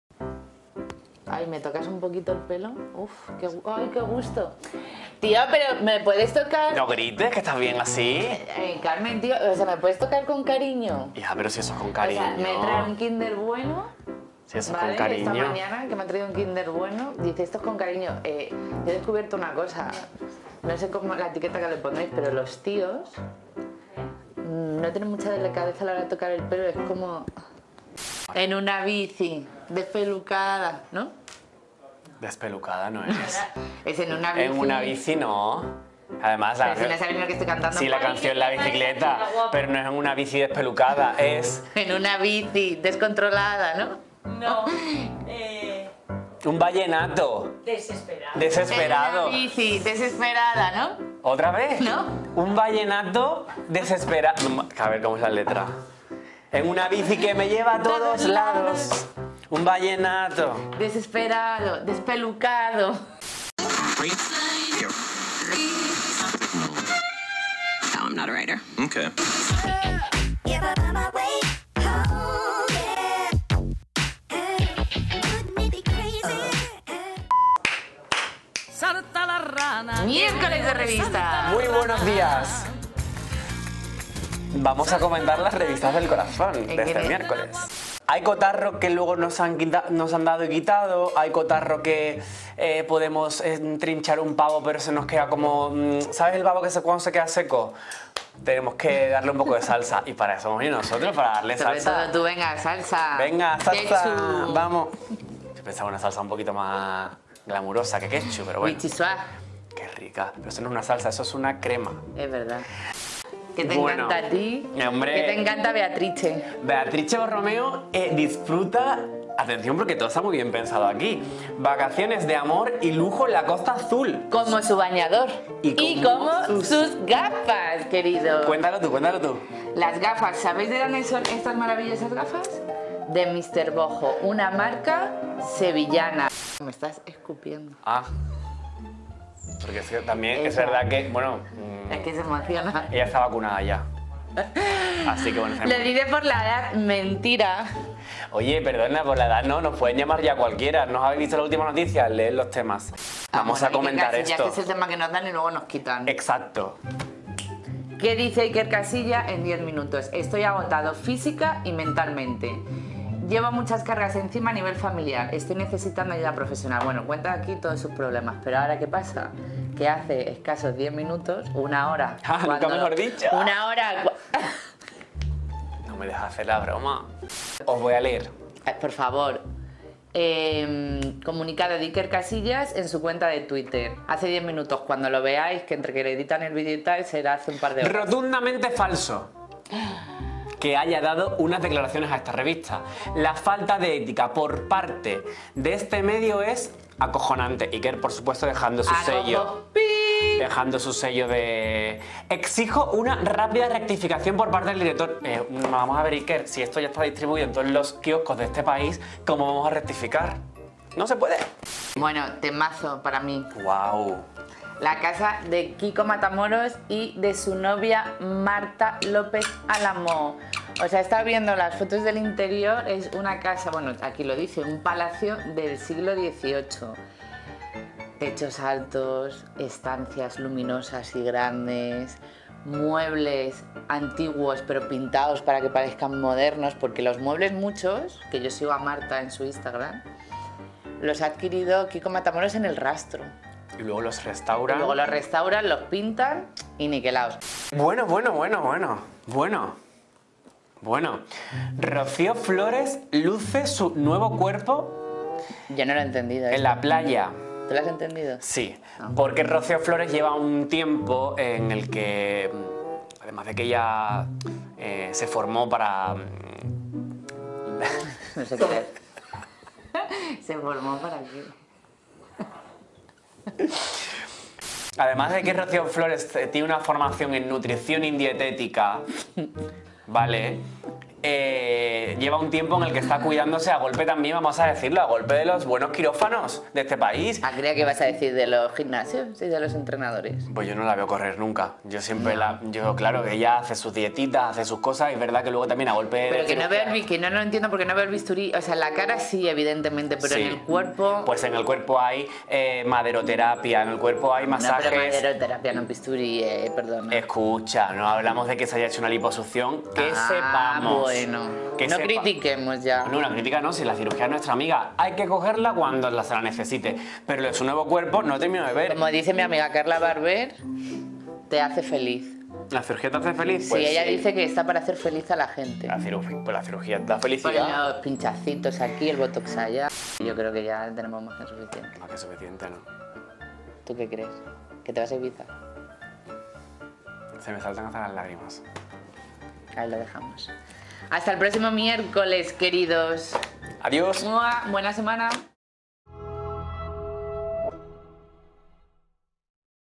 Kiosco rosa, en vídeo: Kiko Matamoros y Marta López muestran su espectacular piso en Madrid
Las revistas del corazón han llegado a los kioscos este miércoles 16 de junio cargadas de las novedades del sector. La revista ¡HOLA! recoge en primera plana a una espectacular Beatrice Borromeo (35 años) con un look de baño que incluye guiño a la moda española, pues la aristócrata luce unas gafas de la marca sevillana Mr. Boho.

Junto a su marido, Pierre Casiraghi (33) y sus hijos, Beatrice ha disfrutado de unas relajadas vacaciones en la Costa Azul. También ha podido escaparse al mar la hija mayor de Antonio David Flores (45), tal y como recoge Semana, que titula: "Rocío Flores (24) luce su nuevo cuerpo en la playa". La madrileña aparece en la portada con la melena al natural, unos short vaqueros y una sencilla parte superior de bikini en color negro.